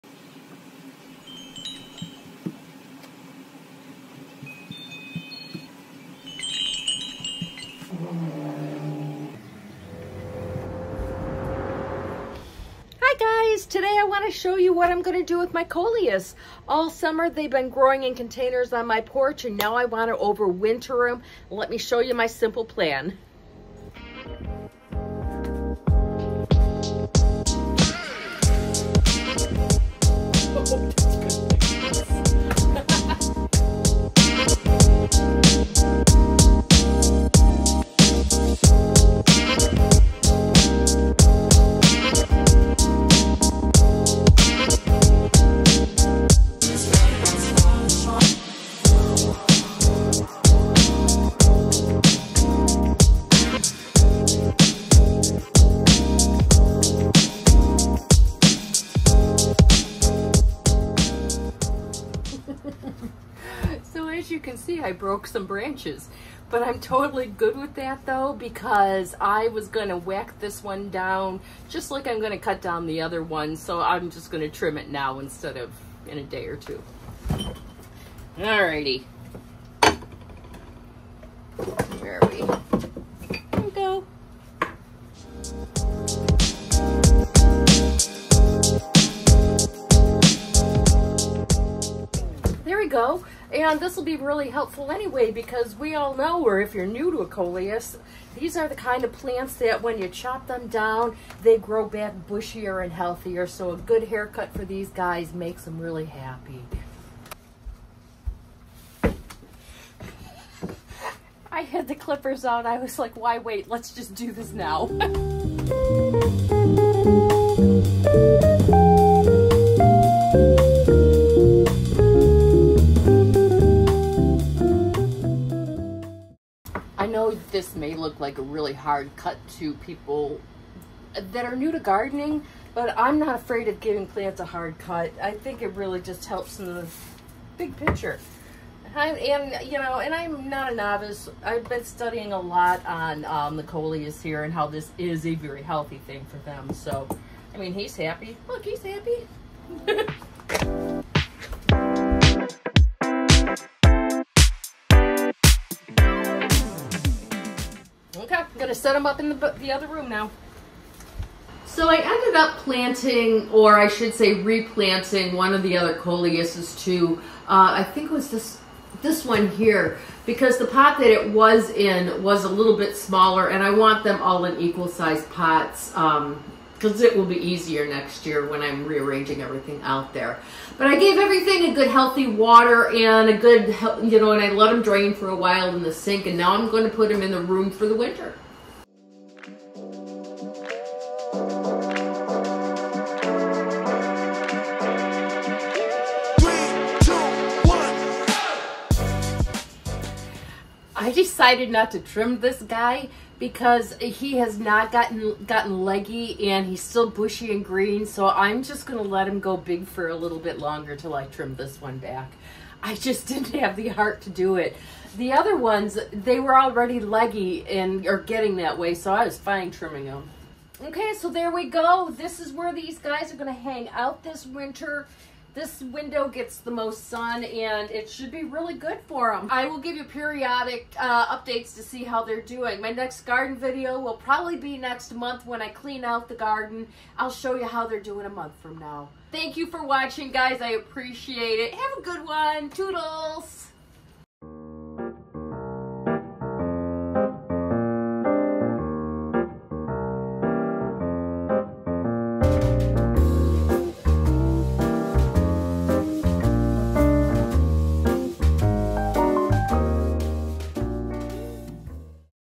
How I Overwinter Coleus Plants, EASY! 9/21/2024
How I Overwinter Coleus  Plants, EASY!  Zone 7 a-b - 9/21/2024

Hello everyone!

My garden is experiencing a long drought this summer and it's rather upsetting to witness.  But I knew when I started this garden that this would be it's challenge and it's been just that.  So I'm going to bypass it this time and focus on my coleus.

My container coleus garden did outstanding this year!  A few of my plants are ready for a cut back, so I thought this would be a great time to clean them up and bring them indoors to overwinter.  I've never overwintered coleus before, so this is an experiment for me ... one I feel I cannot lose!

Join me as I bring them in, repot two plants, hard cut three of the four plants, and get them settled into their new home until next year.

I know to some that I may be jumping the gun, but I'm getting impatient to clean up for the fall.  My lantanas are still strong and blooming as well as my jalapeno plants.  For that I'm happy!

My Tiny Tennessee Home Garden

0:00 Intro
0:24 Repotting
1:03 Cutting Back
2:42 Watering and Draining
3:00 Settling into their Winter Home
4:22 Conclusion

Container Gardens
Coleus Care
Coleus Garden
Overwintering Annuals
Nashville Garden